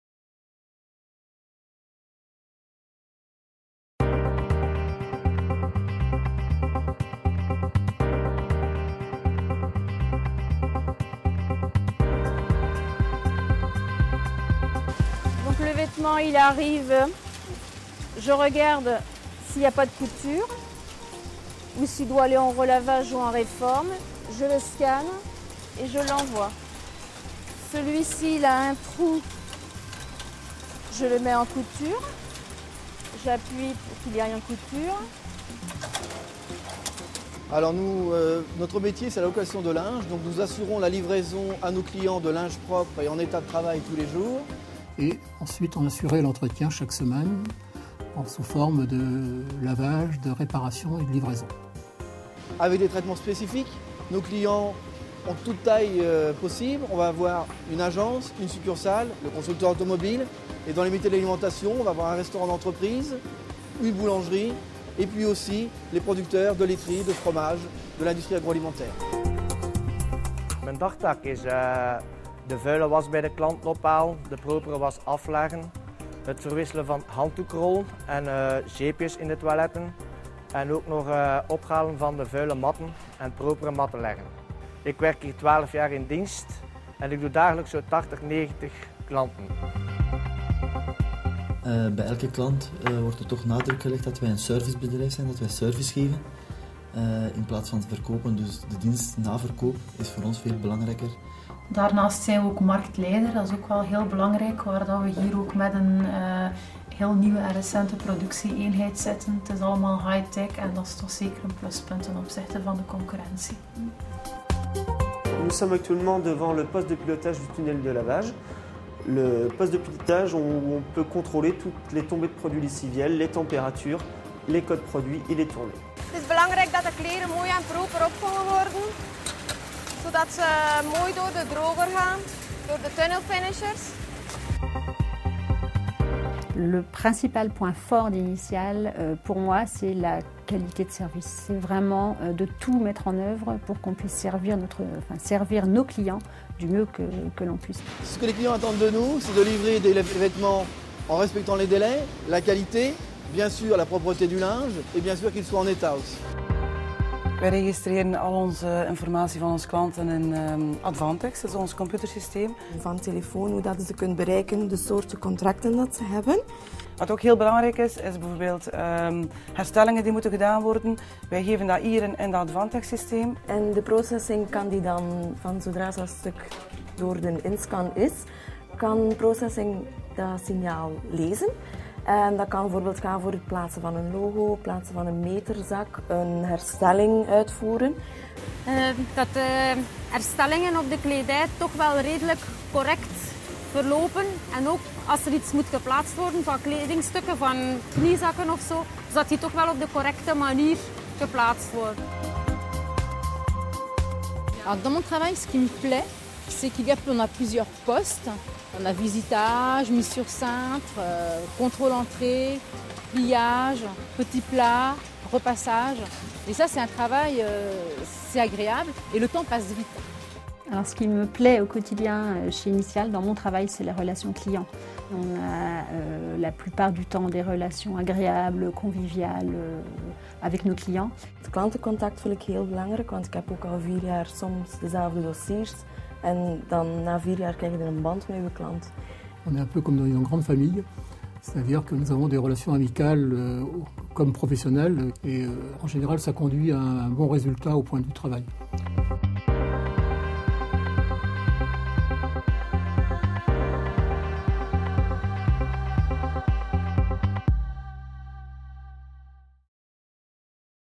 Initial Textiles - Europe
In Europe we specialise in the supply and laundering of work wear, uniforms, clean room uniforms and protective equipment. We offer a linen service for hotels, restaurants and hospitals, floor protection mats, gloves, high-visibility clothing and hand-care products to industry and commerce.